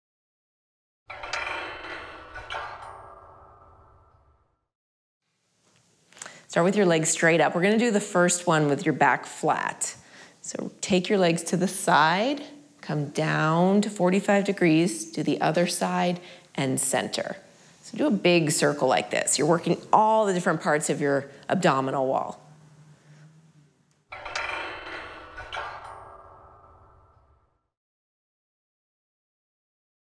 Rebuild Your Abs PostNatal Reformer PREVIEW
/ The Reformer is the perfect place to rebuild your abs after pregnancy because the spring resistance wakes up your muscles faster. Join Molly and her student Elizabeth, who is 6 months postnatal, for a workout that will make your body feel like your own again! Filmed at Pilatesology Studio.